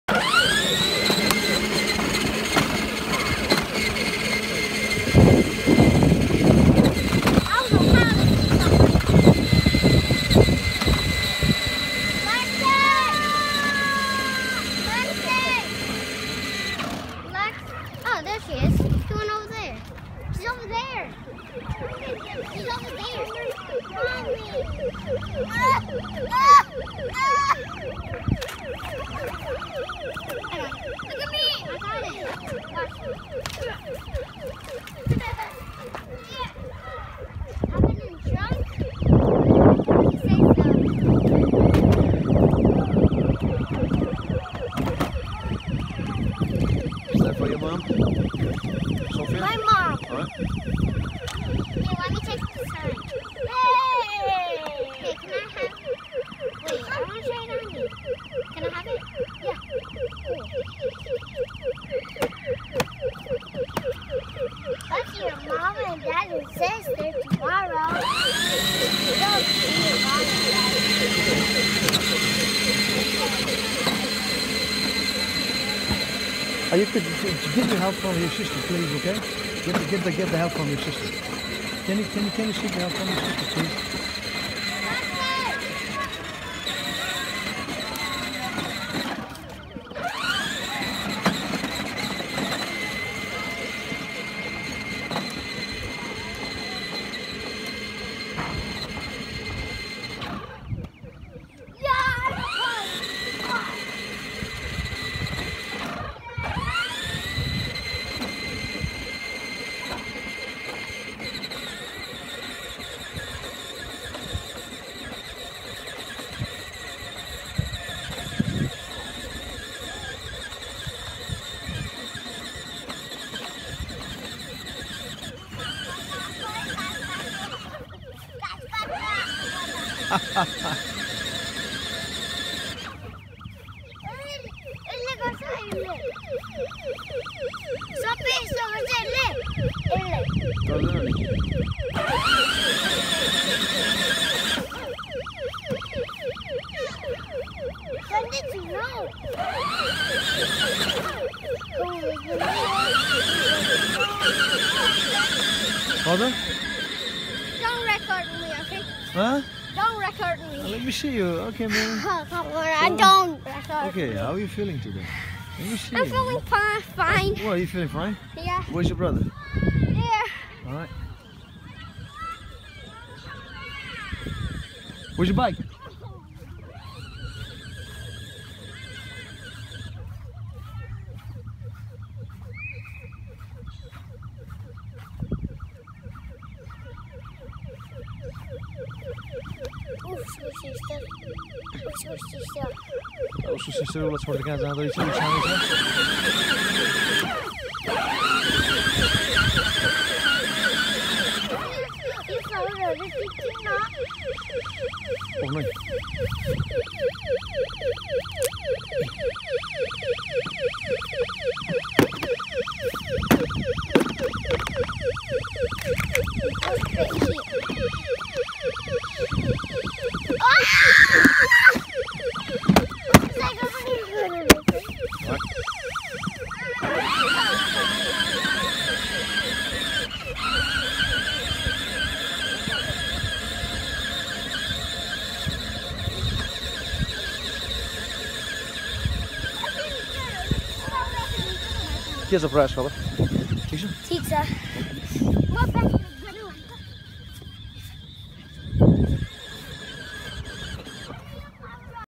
Motorcycle ride
Riding her motorcycle at Jefferson park